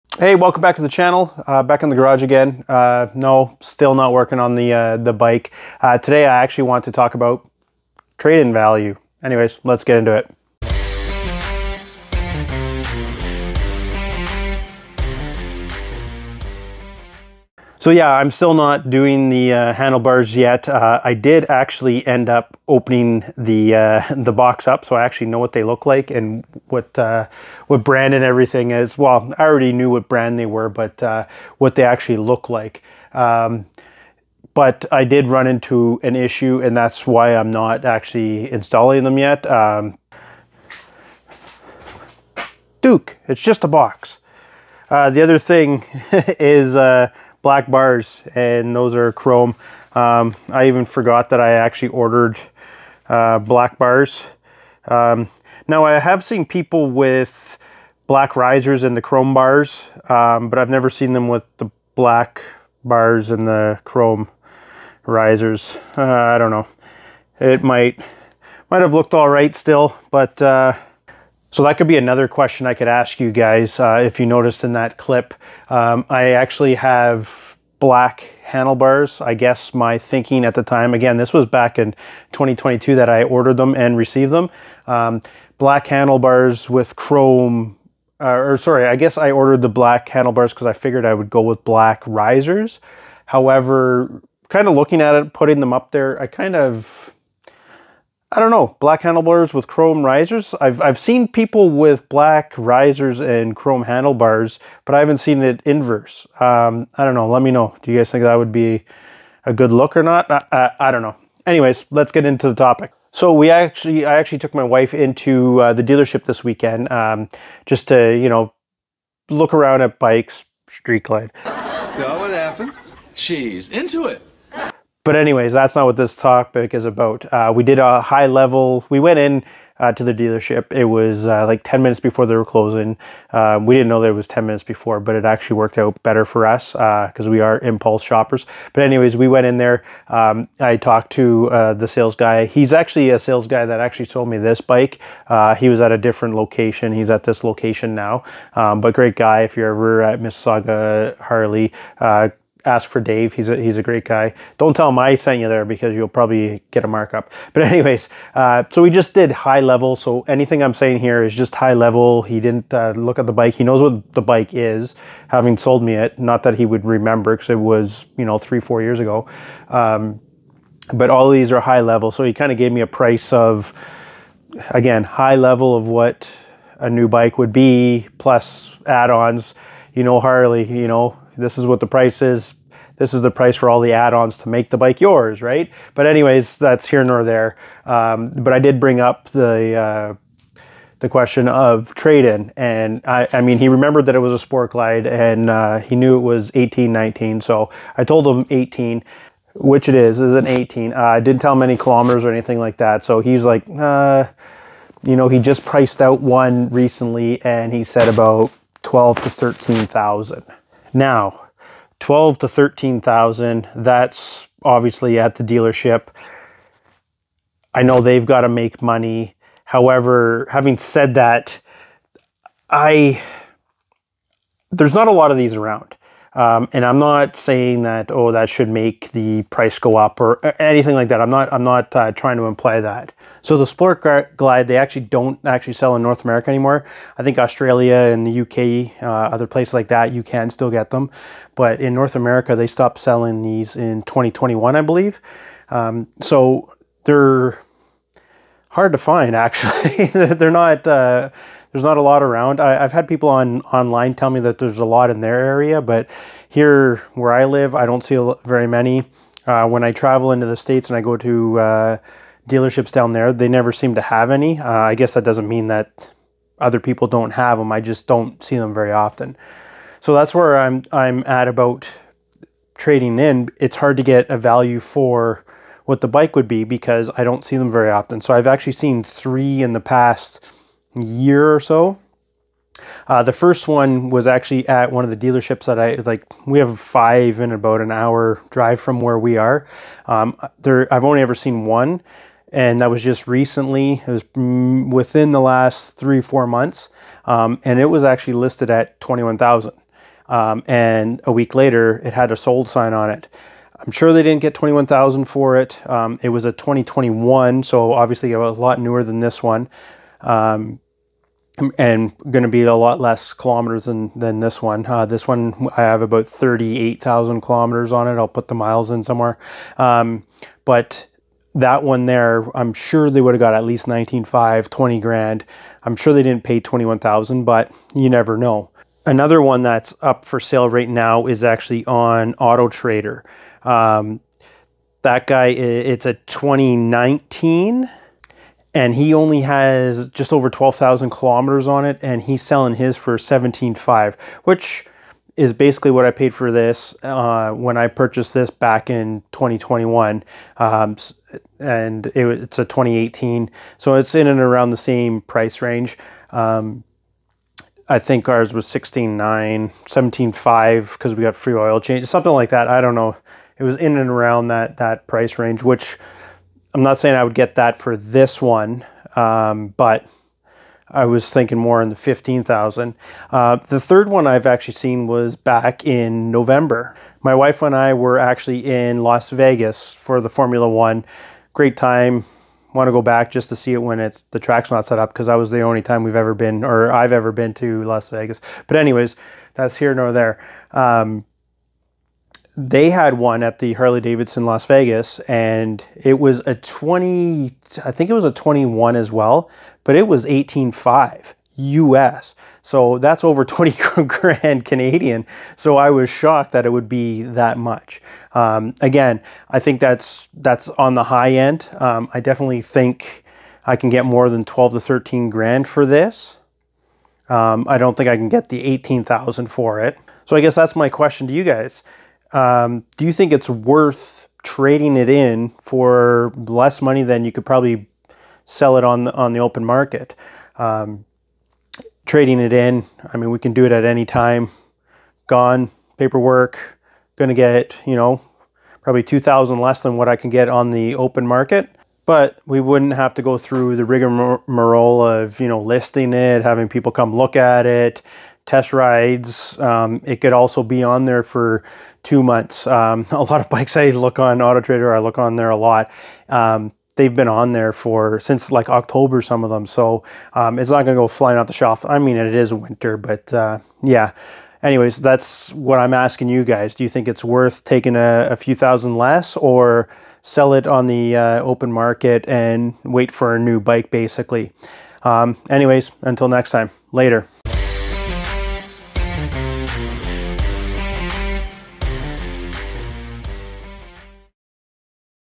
Dealer Trade In Value: Is It Worth It?
My wife and I recently made a pit stop in with a dealership to ask some questions about a new bike. One of the topics we discussed was the value of a trade in for our 2018 Sport Glide. I wanted to see what you think is better, getting less and trading it in so you don't have to go through the hassle of selling it privately, or selling it privately and getting more money is the way to go?